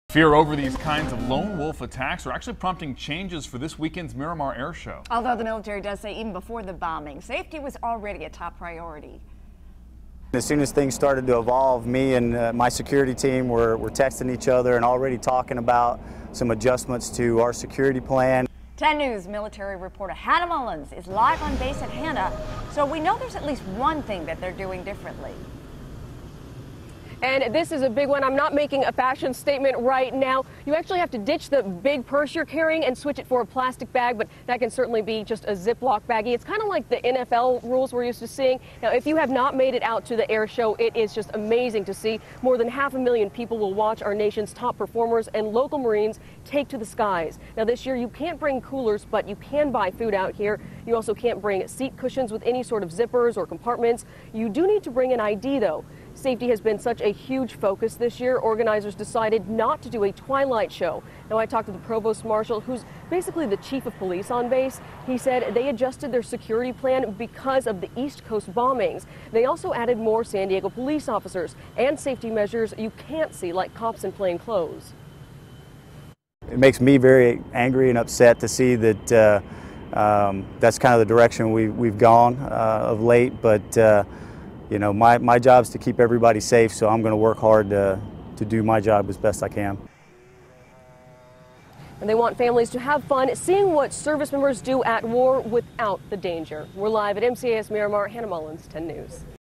Safety crackdown ahead of Miramar Air Show
Bombings in other parts of the country are prompting officials to reconsider security measures at local events.